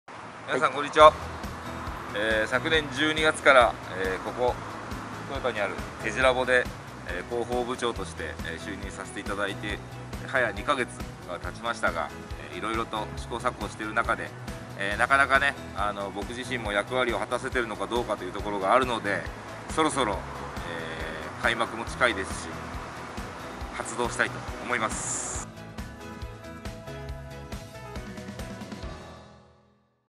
グランパス開幕に向けて杉本恵太が始動
名古屋グランパスのサポーターが集い、世界中のサッカーファンが集まる聖地を作りたい！
と夢を掲げて、豊田スタジアム近くで営んでる韓国料理「テジラボ」の運営スタッフとして就任。
是非、お立ち寄りください。